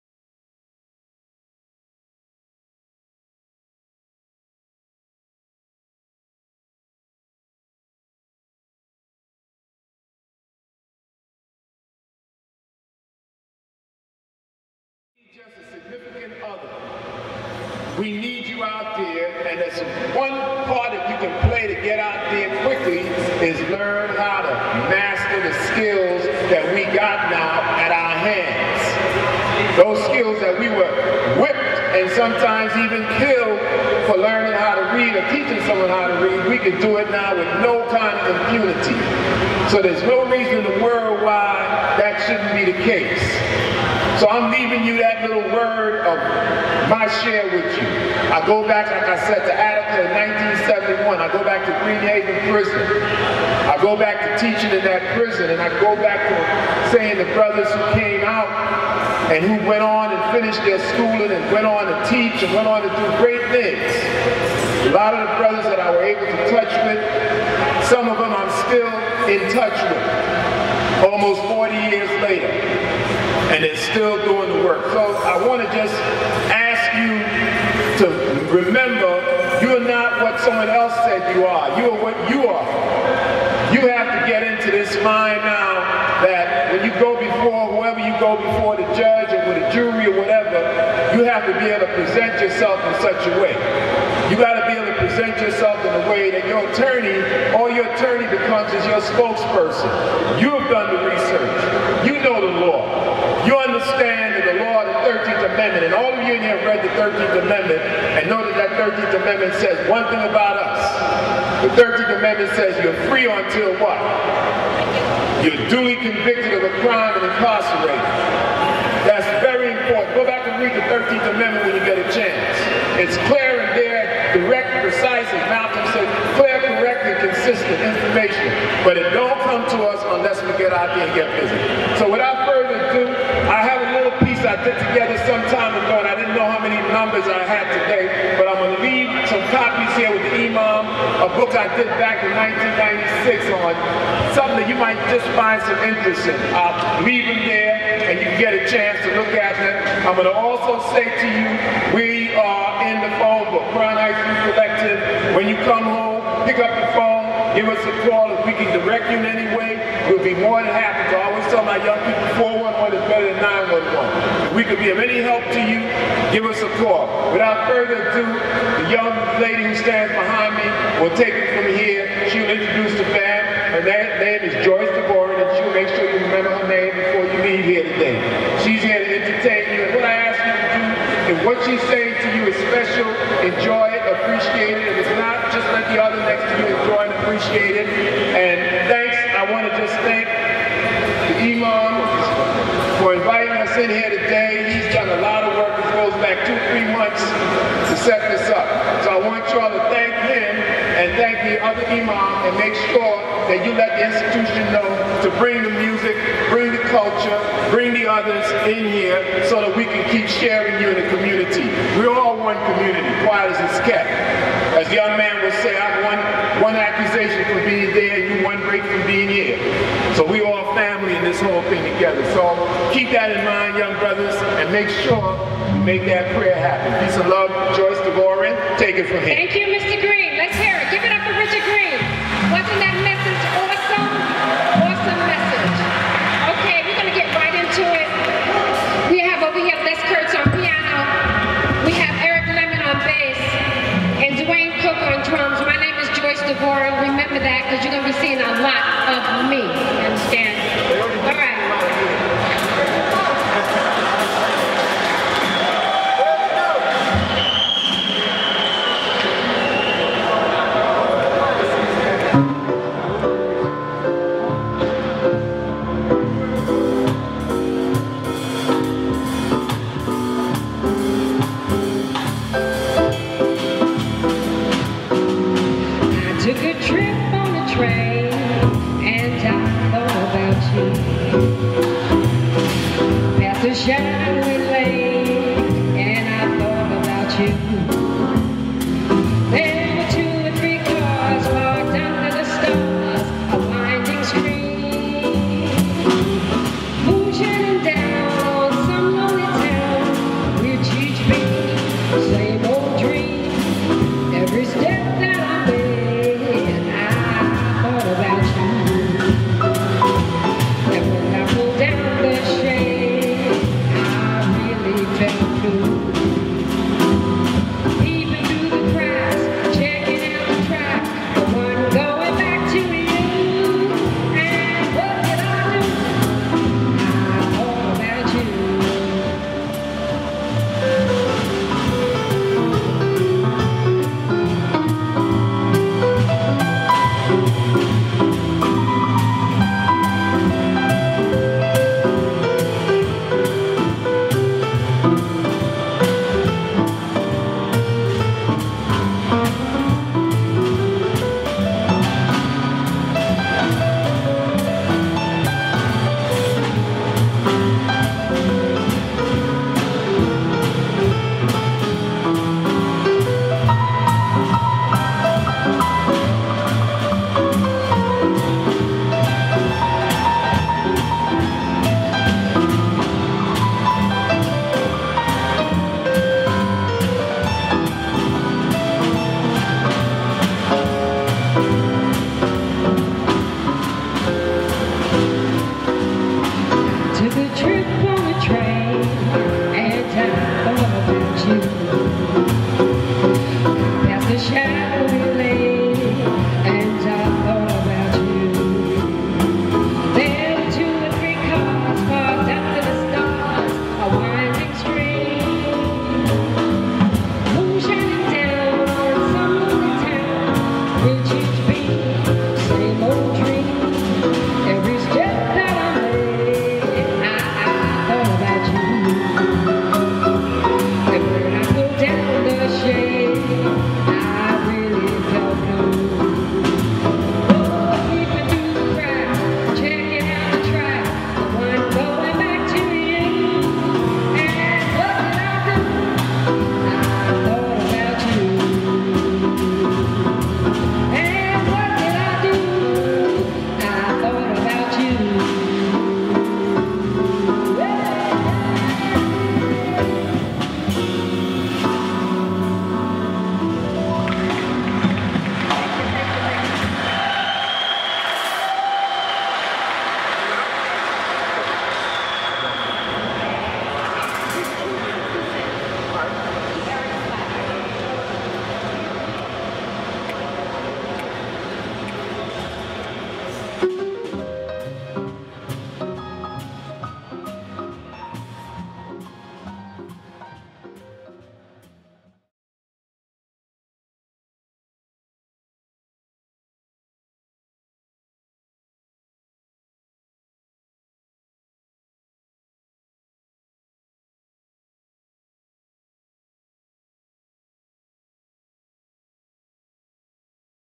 Jazz in the Triangle - JOYCE DAVOREN Live RIKERS ISLAND HQ
Joyce Davoren performs at Rikers Island. Open message by Richard Green.